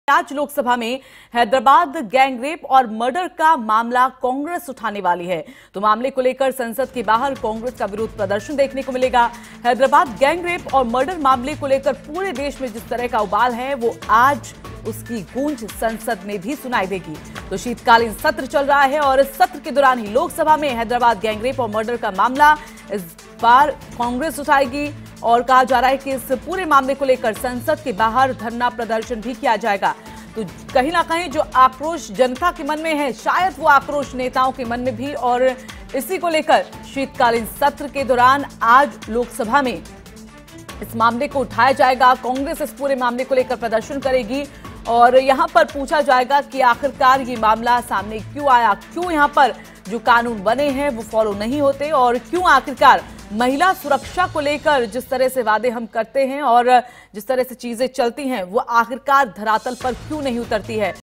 आज लोकसभा में हैदराबाद गैंगरेप और मर्डर का मामला उठाएगी कांग्रेस
आज लोकसभा में हैदराबाद गैंगरेप और मर्डर का मामला उठाएगी कांग्रेस….मामले को लेकर संसद के बाहर कांग्रेस का विरोध प्रदर्शन….हैदराबाद गैंगरेप और मर्डर मामले को लेकर पूरे देश में सियासी उबाल